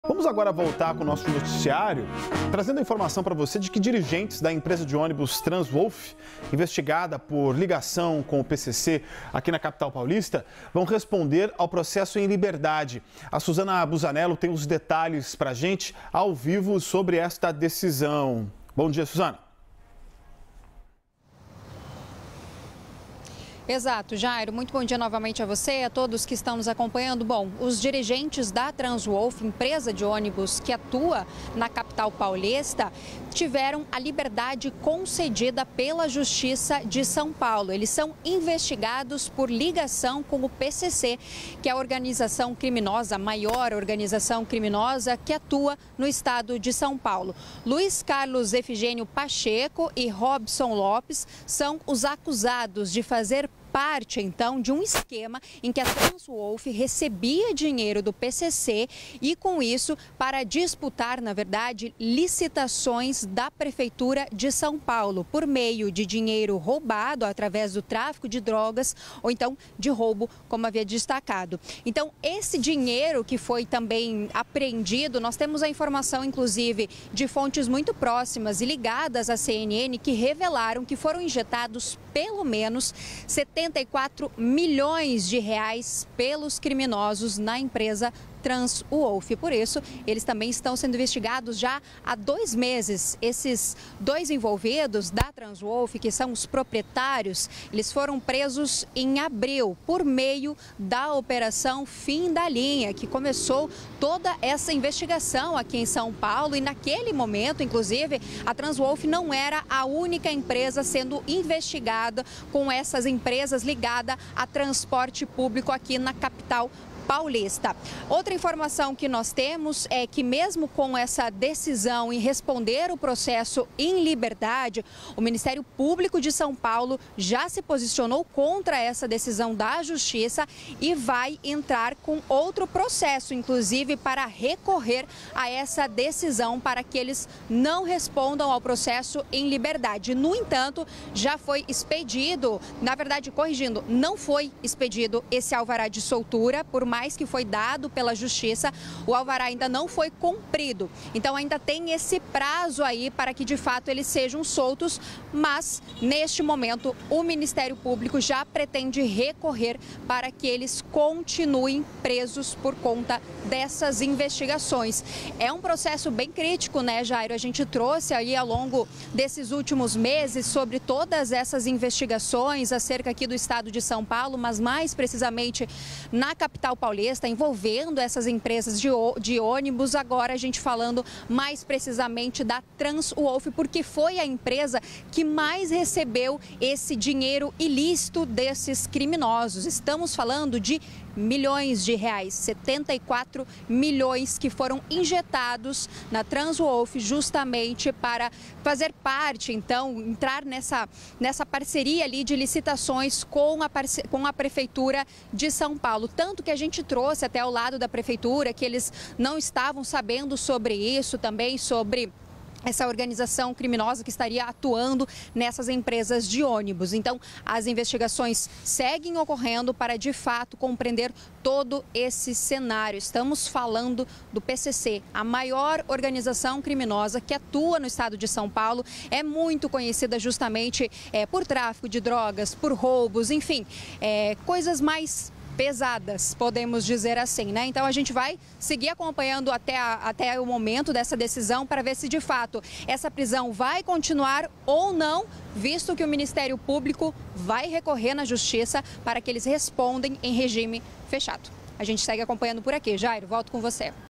Dirigentes da Transwolff vão responder em liberdade | LIVE CNN
Dirigentes da Transwolff, empresa de ônibus que opera na capital paulista e é investigada por ligação com o Primeiro Comando da Capital (PCC), tiveram liberdade concedida pela Justiça de São Paulo. Luiz Carlos Efigênio Pacheco e Robson Flares Lopes são acusados de fazer parte de esquema que recebia dinheiro do PCC para disputar licitações da Prefeitura de São Paulo.